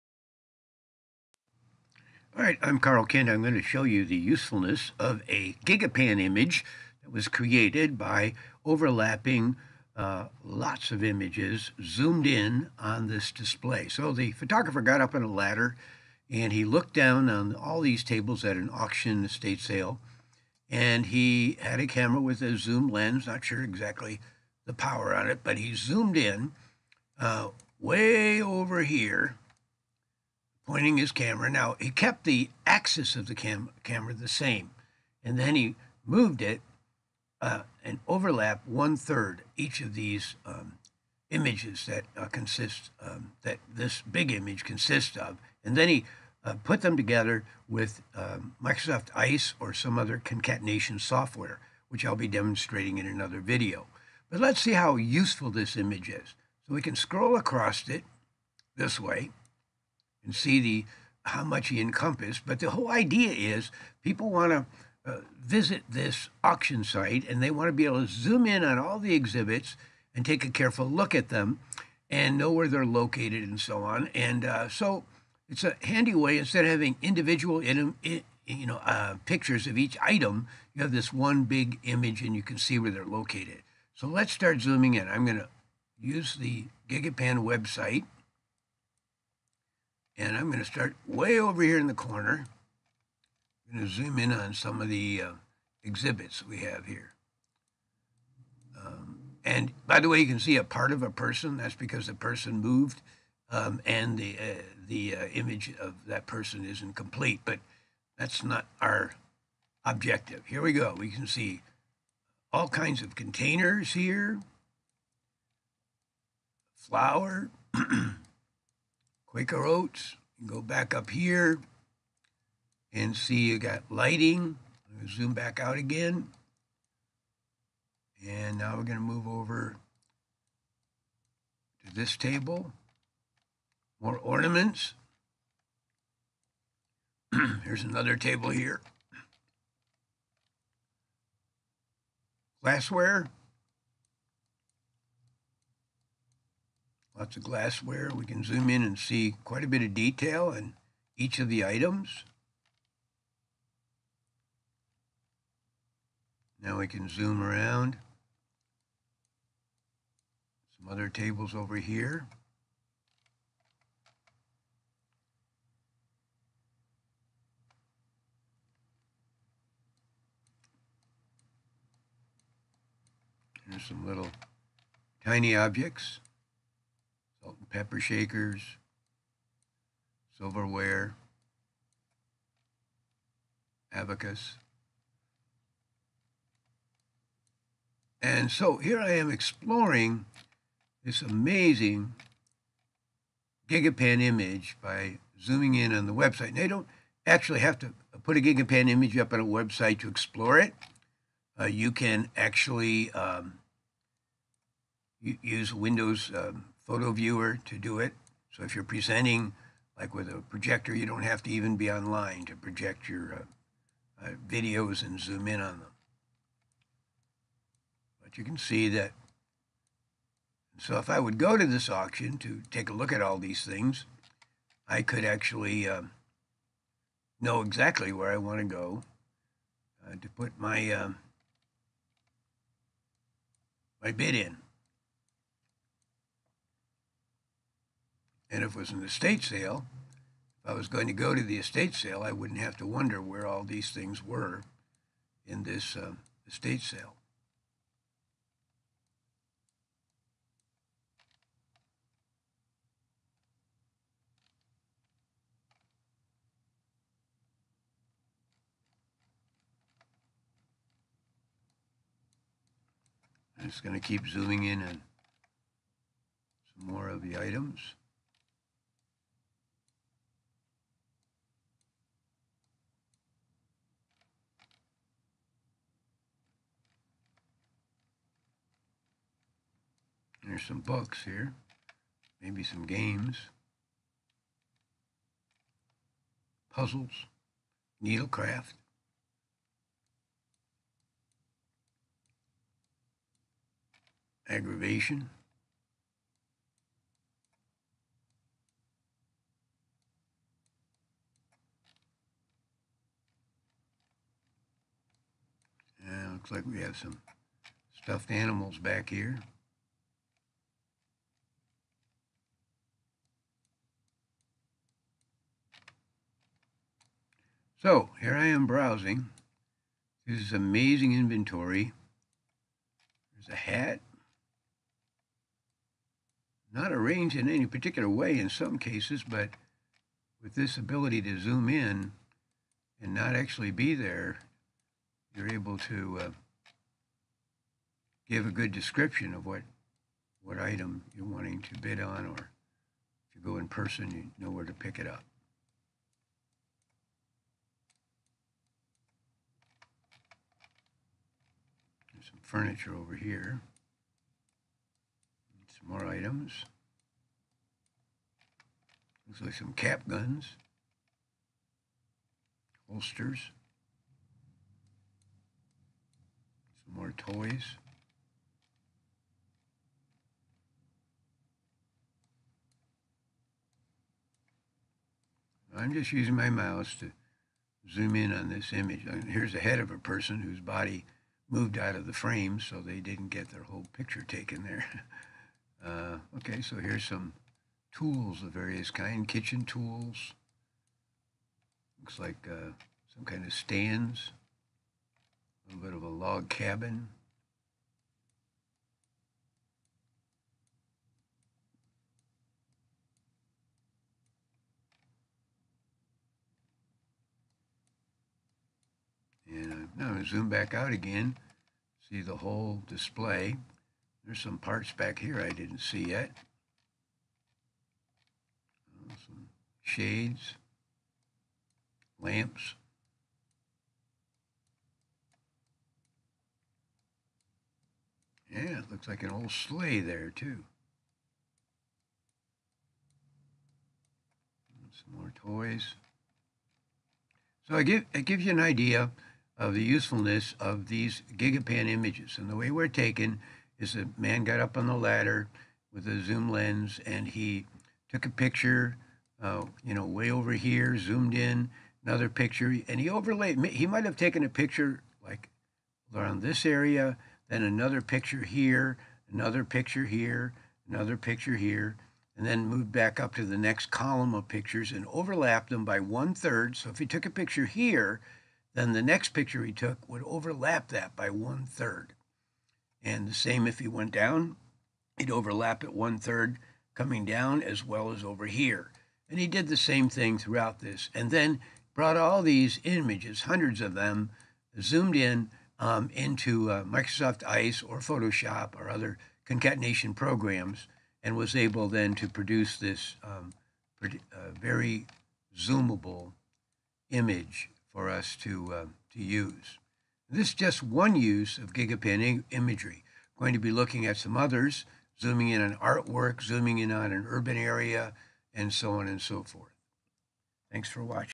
Exploring a Gigapan Image at an auction
This video is a part of a workshop on how to create gigapan images and hwo to use them and how useful they can be in business, law,, science, real estate, criminology, social gatherings and much more.